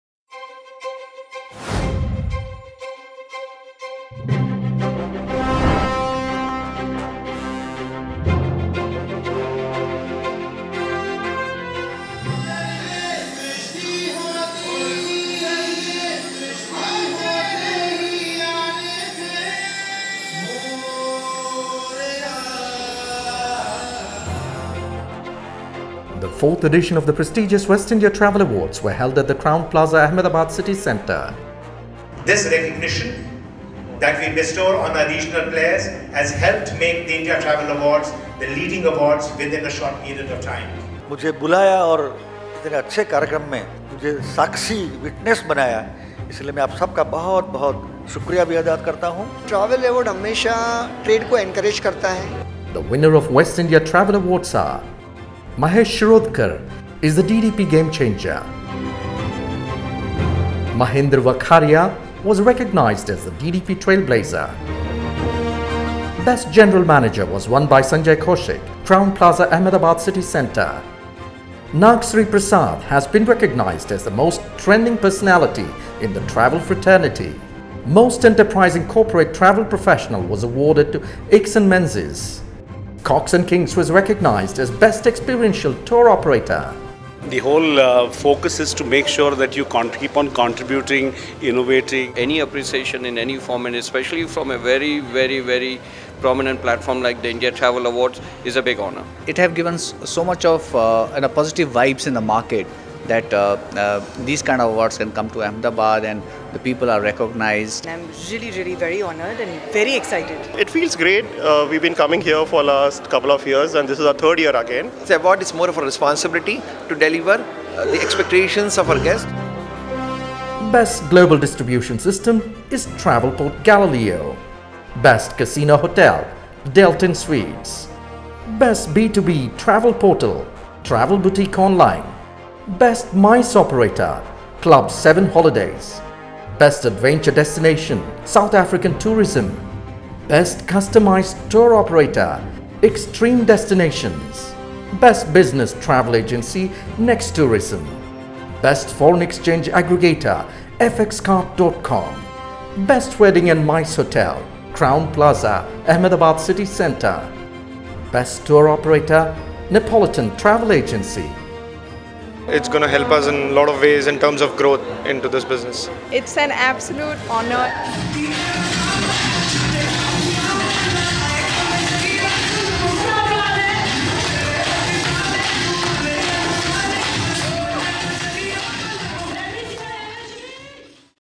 West India Travel Award 2017 (Part 1)
The 4th edition of the Prestigious west India Travel Awards were held at the Crowne Plaza Ahmedabad City Centre . Recognising and encouraging people from different regions of India is the Main ethos behind the very popular India Travel Awards

- MAHESH SHIRODKAR is the  DDP Game Changer
- MAHENDRA VAKHARIA was recognised as the DDP Trailblazer
- Best General Manager was won by SANJAY KAUSHIK , Crowne Plaza Ahmedabad City Centre
- NAGSRI PRASAD has been recognised as the most trending Personality in the travel fraternity
- Most Enterprising Corporate Travel Professional was awarded to IKSON MENEZES
- Cox and Kings was recognised as Best Experiential Tour Operator
- Best Global Distribution System is Travelport Galileo
- Best Casino Hotel - Deltin Suites
- Best B2B Travel Portal - Travel Boutique Online
- Best  MICE Operator - Club 7 Holidays
- Best Adventure Destination - South African Tourism
- Best Foreign Exchange Aggregator - FxKart.com
- Best Wedding and MICE Hotel - Crowne Plaza Ahmedabad City Centre
- Best Customized Tour Operator - Xtreme Destinations.
- Best Business Travel Agency - Nextourism
- Best Tour Operator - Napolitan Travel Agency.

We Interviewed

- SanJeet, Mentor, India Travel Awards
- BHUPENDRASINH MANUBHA CHUDASAMA, Cabinet Minister- Revenue, Education (Primary, Secondary and Adult), Higher and Technical Education, Legislative and Parliamentary Affairs, Govt. of Gujarat
- NIRAV MUNSHI , Manager -Tours & Travels, Gujarat Tourism
- MAHESH SHIRODKAR,   Managing Director, Tamarind Global
- MAHENDRA VAKHARIA,  Managing Director, Pathfinders Holidays
- SANJAY kAUSHIK,  Area General Manager-West India at Inter Continental Hotels, Crowne Plaza Ahmedabad
- NAGSRI PRASAD SASHIDHAR, Head- Outbound Holidays, Mercury Travels
- IKSON MENEZES, Senior Manager- Travel Related Services, Mahindra & Mahindra
- SANJEEV CHHAJER, Vice President, Cox & Kings
- CHINTAN KAYANI, Director, Xtreme Destinations
- BANAFSHE PASHOOTANIZADEH, Partner-Business, Napolitan Travel Agency